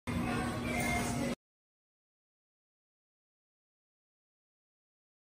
Minka Aire Roto ceiling fan at a pizza restaurant
This was in New York. Enjoy